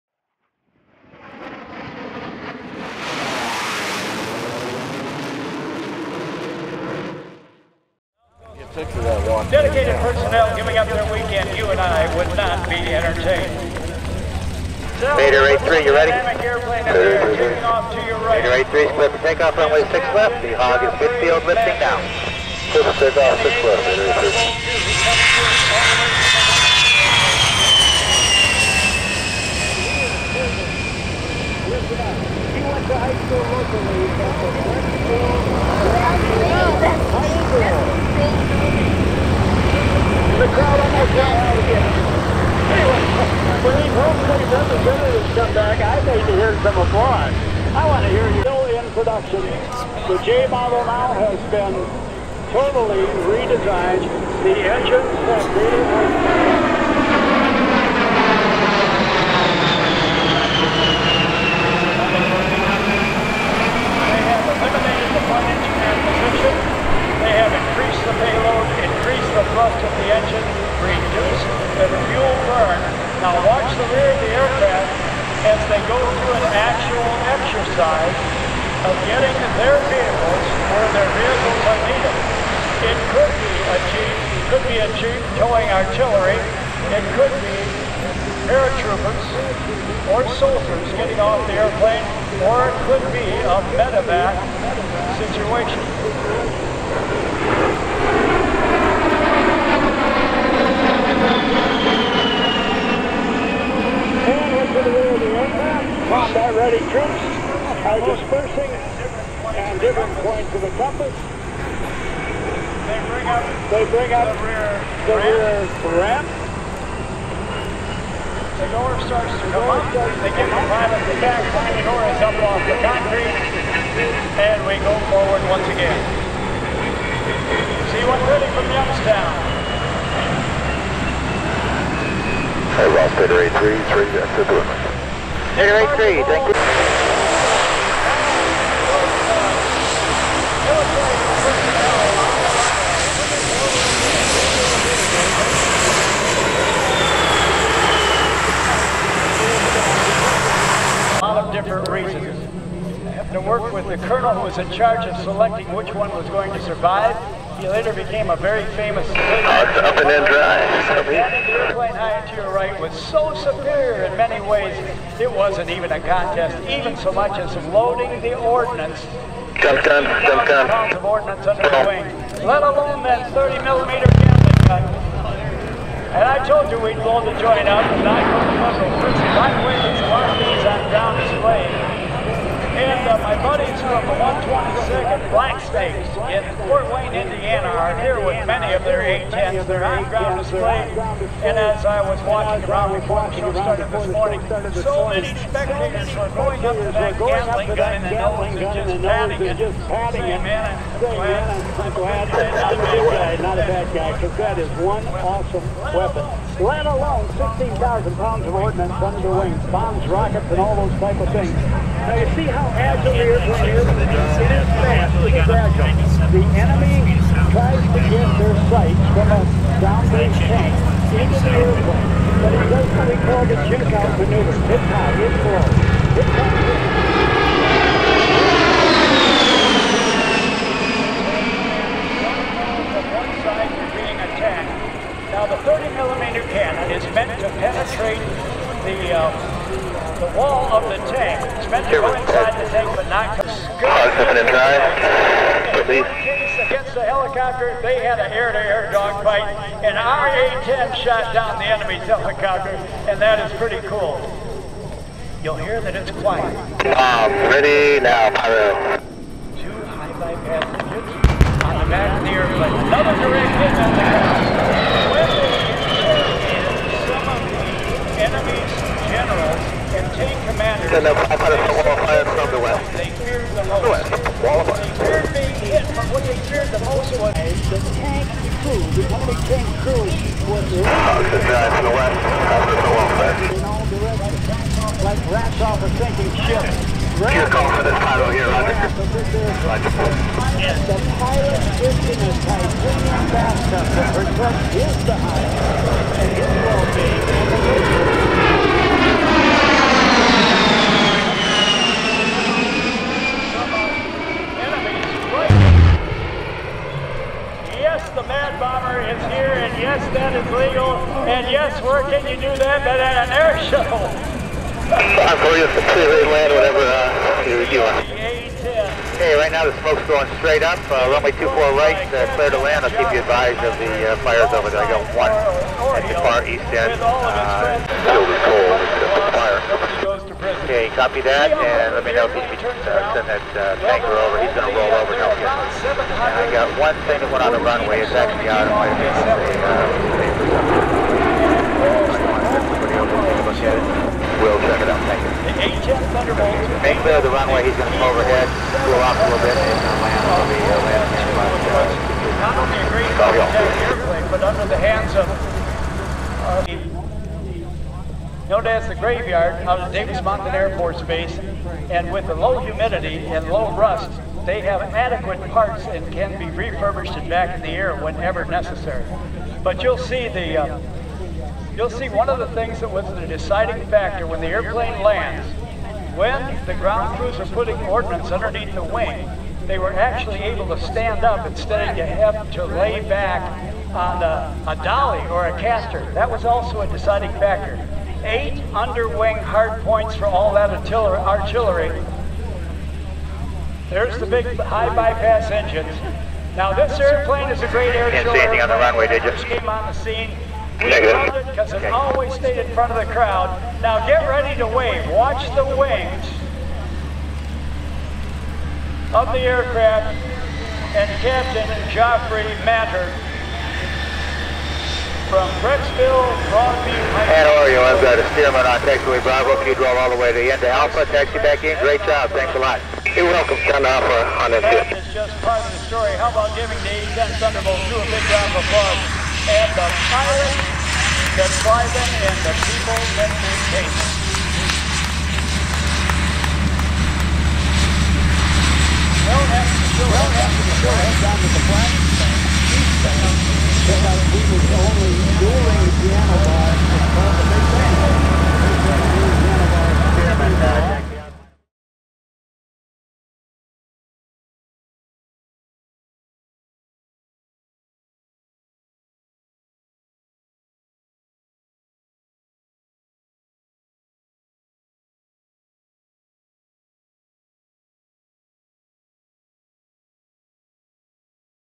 A-10 Warthog Field Attack Demo w/ Pyro! - Cleveland National Airshow 2018
An A-10 Thunderbolt II performs a field attack demonstrating at the 2018 Cleveland National Airshow. Watch for the pyro on the ground simulating strafing and bombing passes, including a wall of fire!